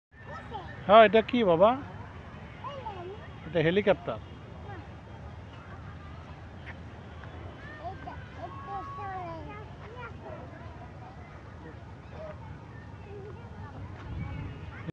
তুবার বিমান দেখা
টিভিতে প্লেন দেখতেই তুবা মা মনির ভালো লাগে। এবার সে বিমান বাহিনীর জাদুঘরে বিমান দেখলো। মহান আল্লাহ আমার মেয়েকে অনেক বড় হিসাবে প্রতিষ্ঠিত করুন। আমি তার প্রতিটা মূহুর্তের সফলতা কামনা করি।